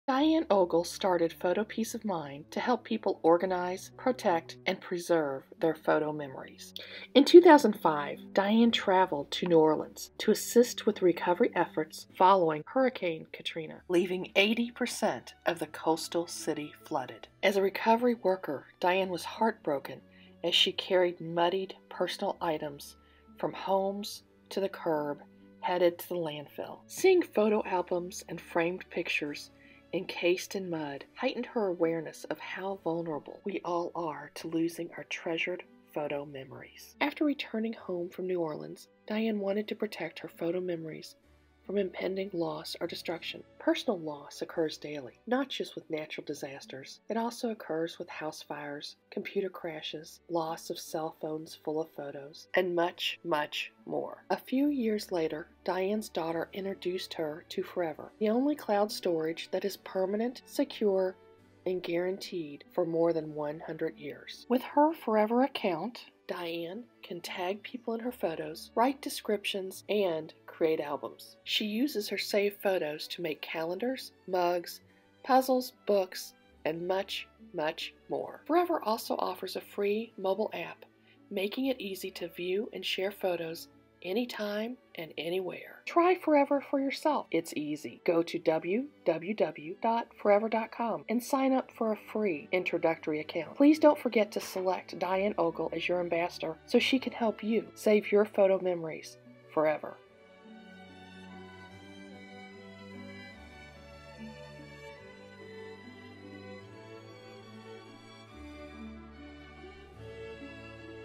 Diane Ogle's Forever® Story
Diane Ogle shares her story of why she chose Forever® cloud storage to preserve her photo memories.